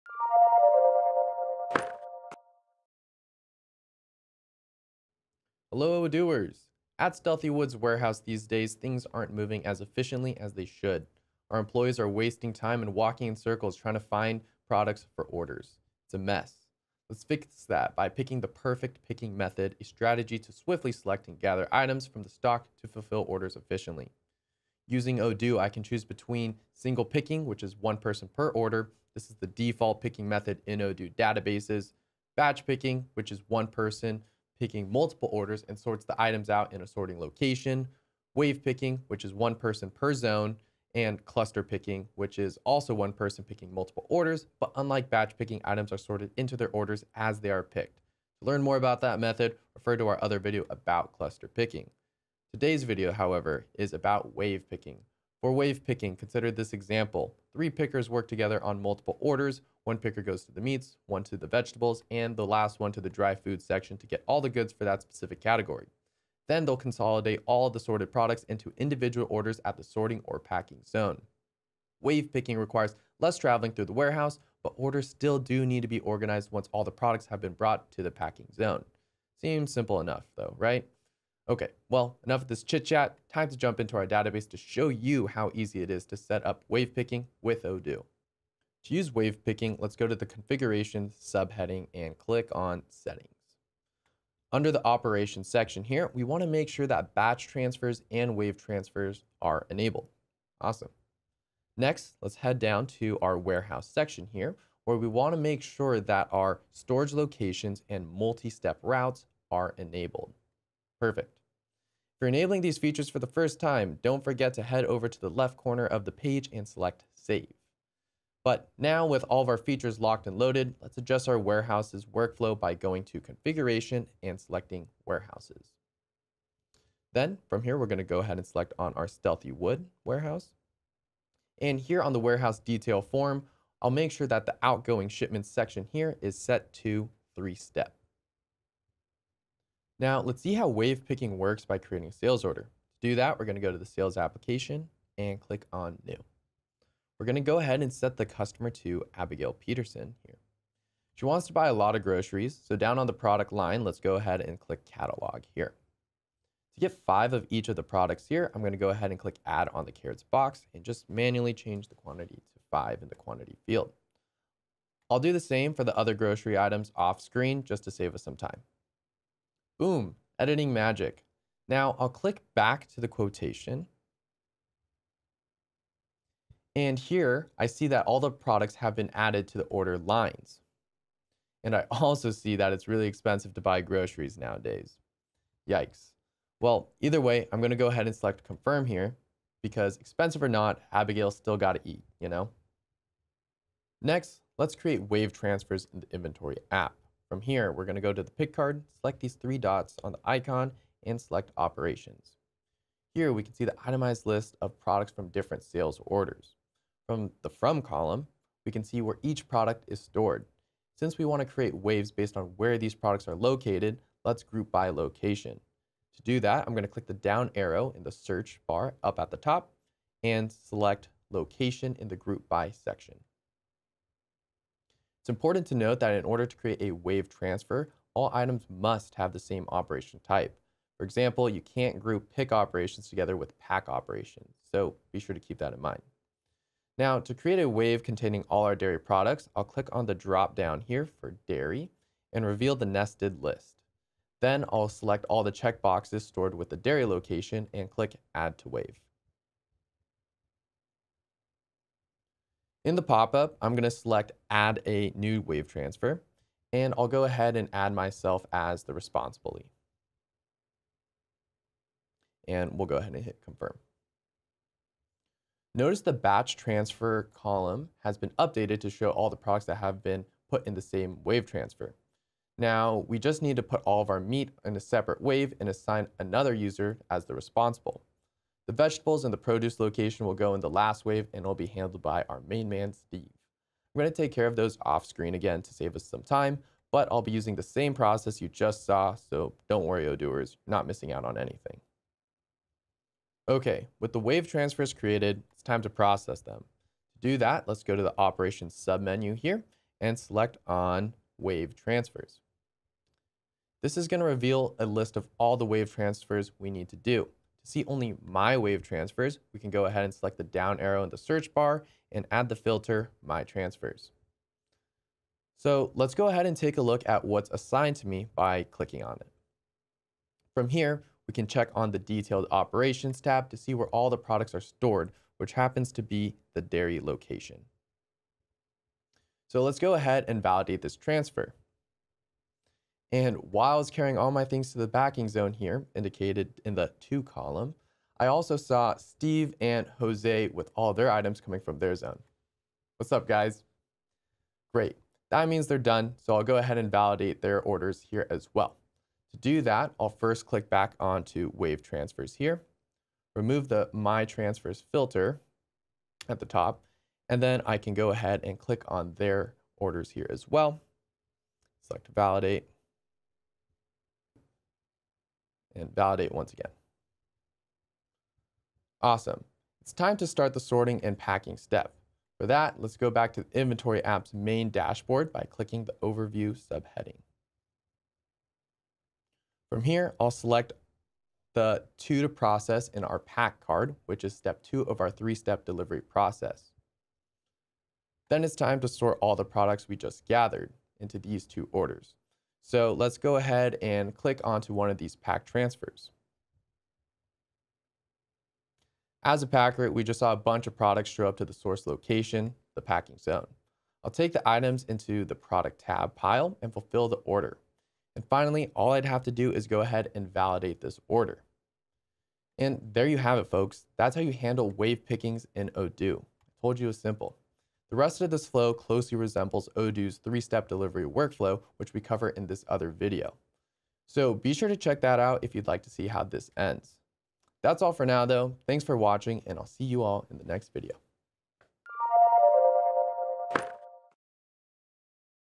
Picking Methods - Wave Picking | Odoo Inventory
In this video, learn about the various picking methods available, and examples of when to use them, to efficiently run your warehouses.

0:00 - Introduction
1:28 - Set up
2:18 - Create Sales Order
3:13 - Create Wave
4:53 - Process Wave Transfers
6:08 - Sort Products into Orders
6:52 - Conclusion